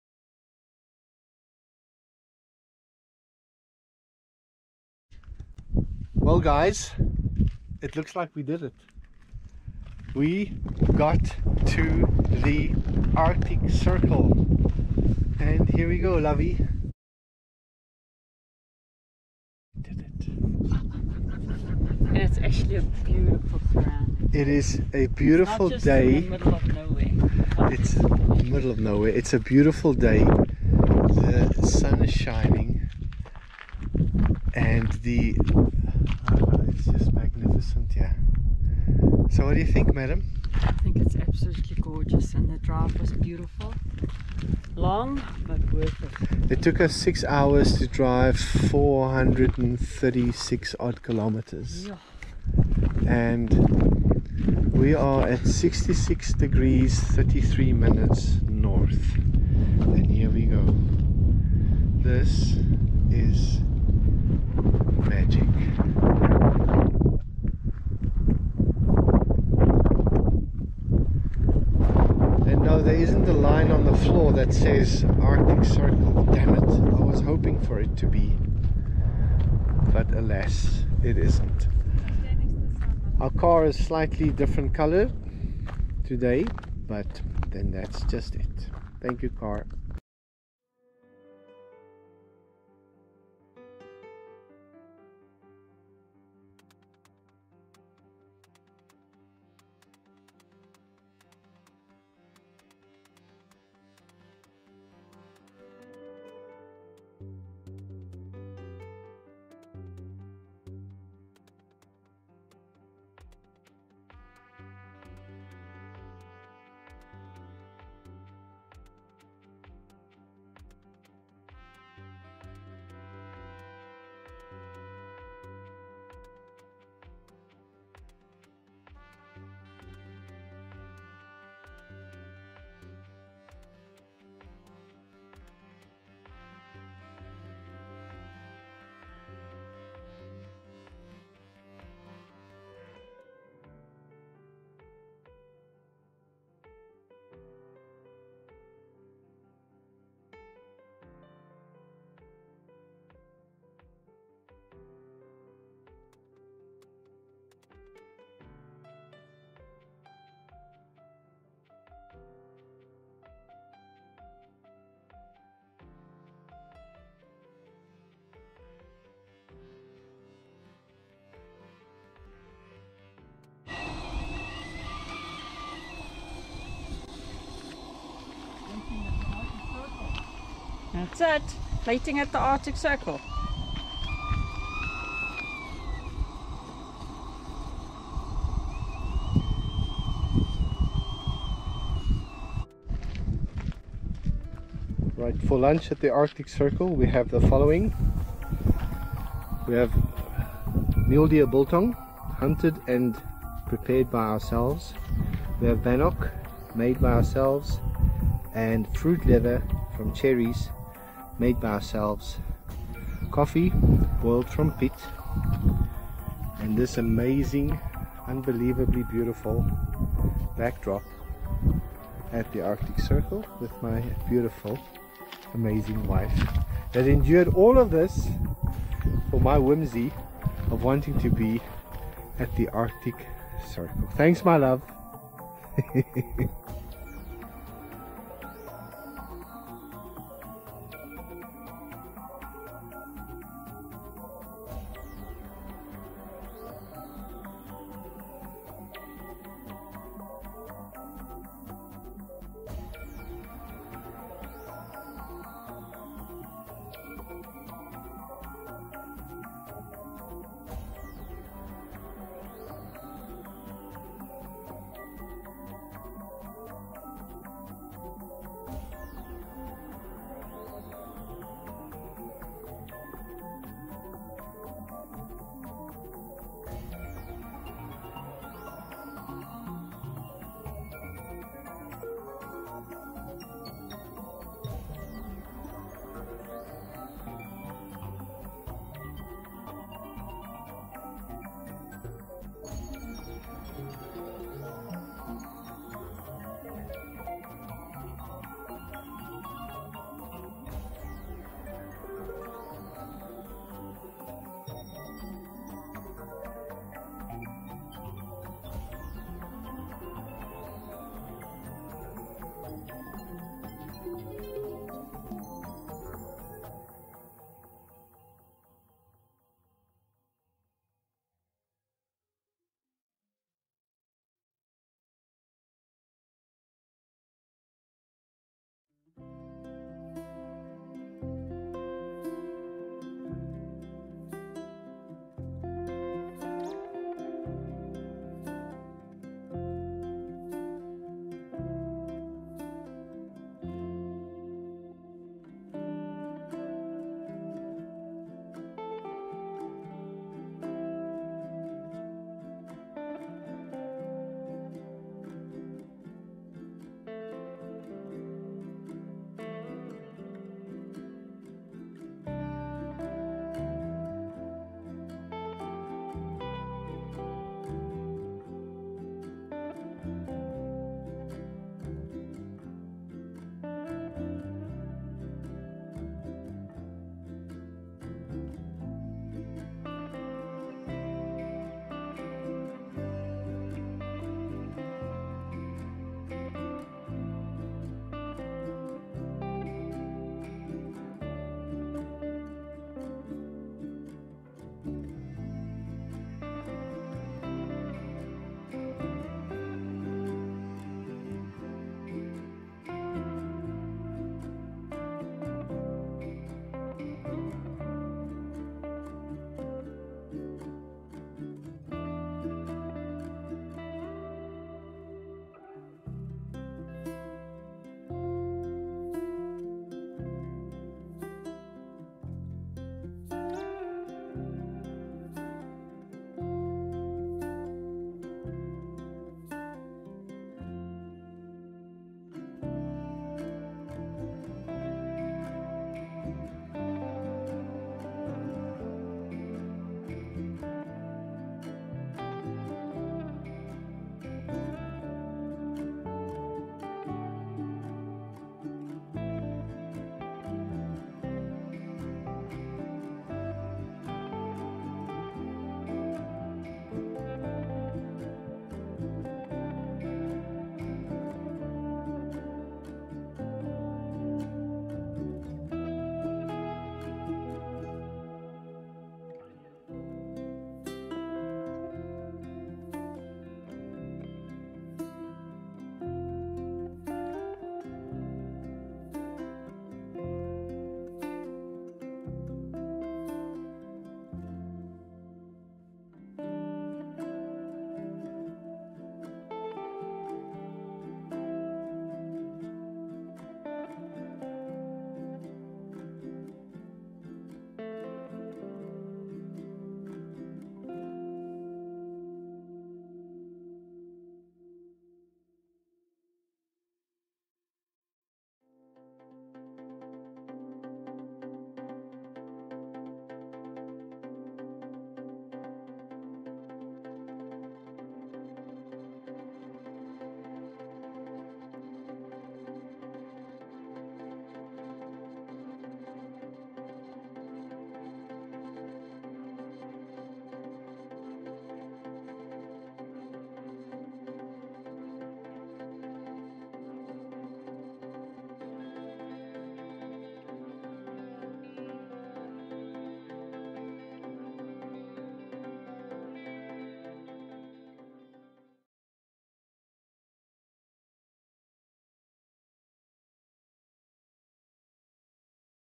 COLT #3 Day 13 Part 2
COLT #3 Day 13 Part 2. Journey to the Yukon.  Touching the Arctic Circle.